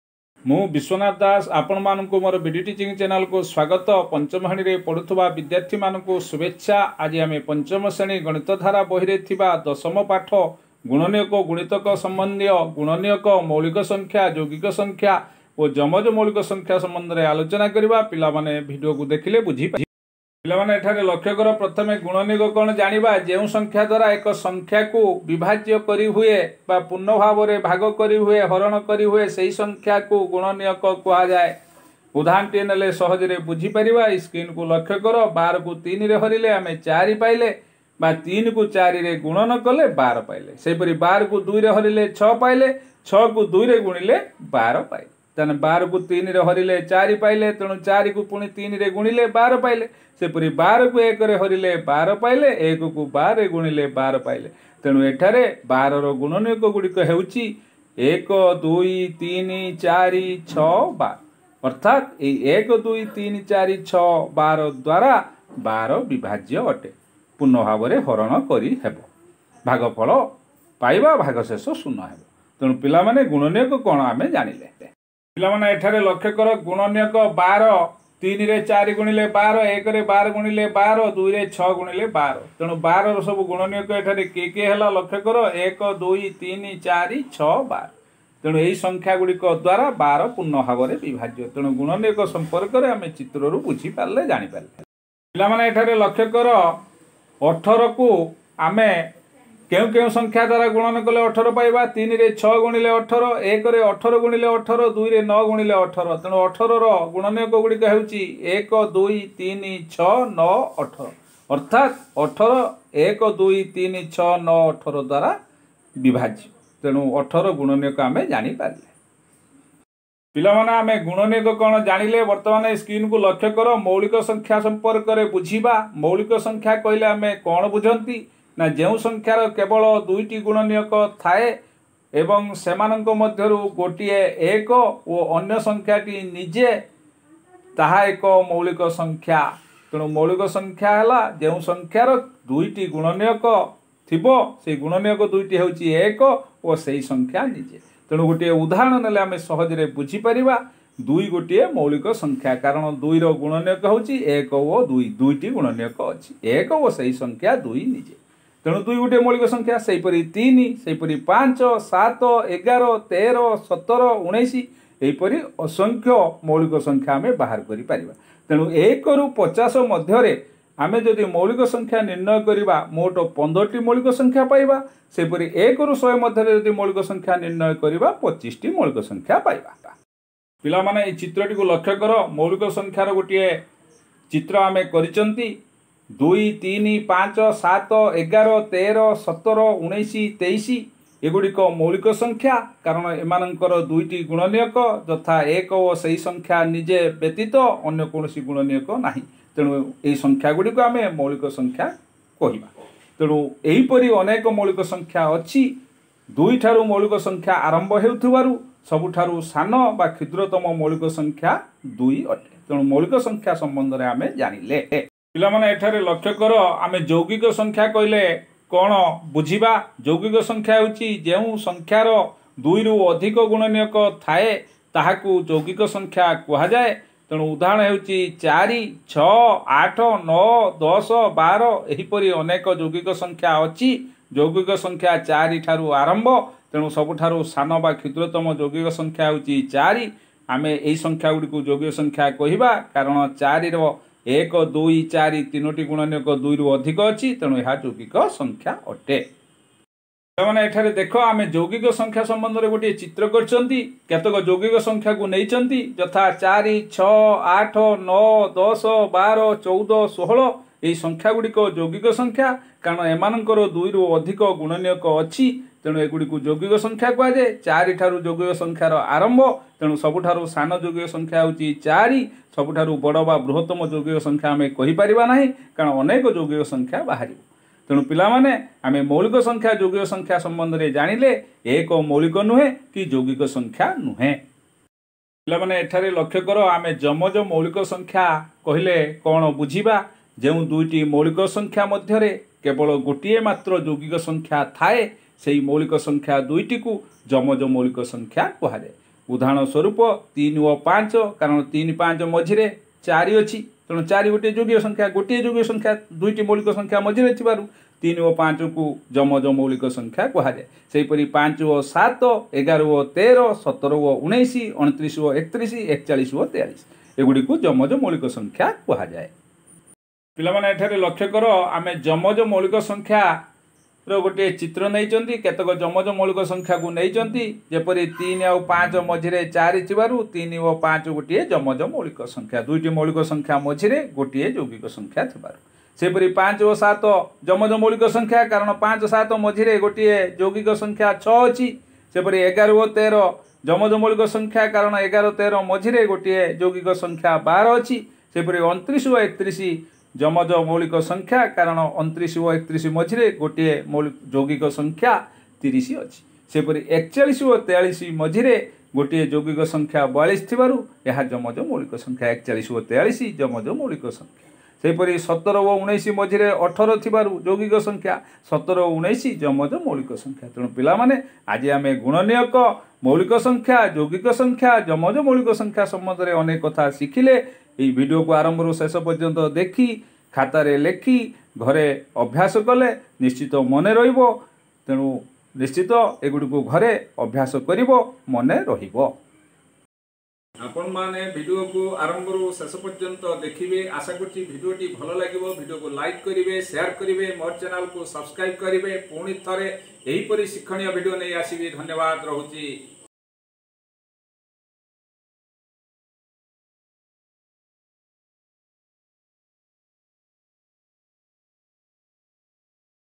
5 Class Math -Gunaniyakaand Gunitaka (ଗୁଣନୀୟକ ଓ ଗୁଣିତକ )(Odia Medium )
5 Class Math ଗୁଣନୀୟକଓ ଗୁଣିତକ (ଗୁଣନୀୟକ, ମୌଳିକ ସଂଖ୍ୟା, ଯୌଗିକ ସଂଖ୍ୟା, ଯମଜ ମୌଳିକ ସଂଖ୍ୟା )Odia Medium